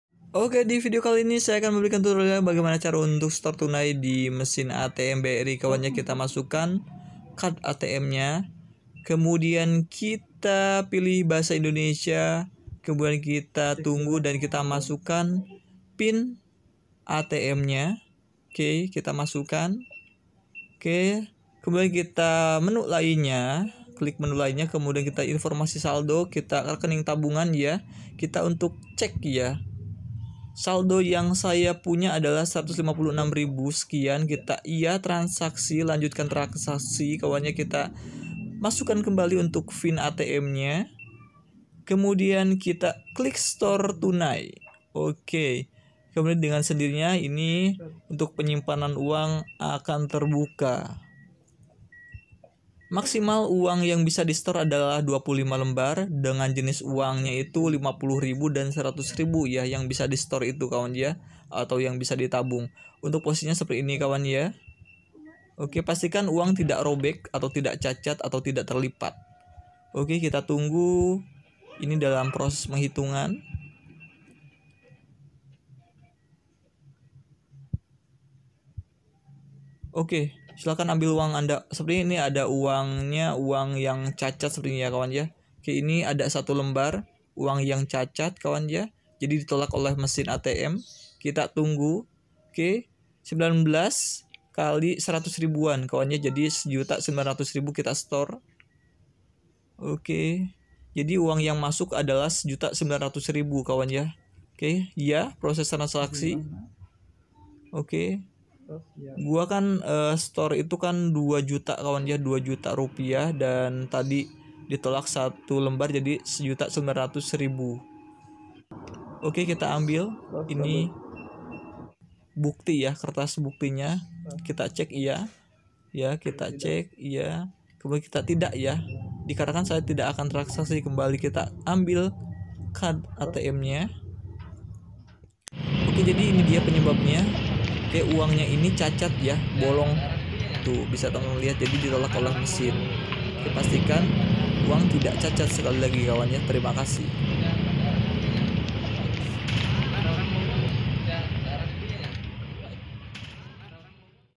Cara setor tunai di ATM BRI / Nabung lewat mesin ATM
Cara setor tunai di ATM Bank BRI, saya pertama kali untuk menyambung uang lewat mesin ATM atau bisa disebut setor tunai. Untuk pemilihan menu setor tunai di ATM sangat mudah. Tetapi kita harus hati - hati agar setor tunai berjalan lancar pastikan kita harus cek uang kita yang akan di tabung atau di masukan ke mesin ATM jangan sampai uang kalian terlipat.

Untuk setor tunai ATM BANK BRI  bisa kalian ikuti langkah
 langkah berikut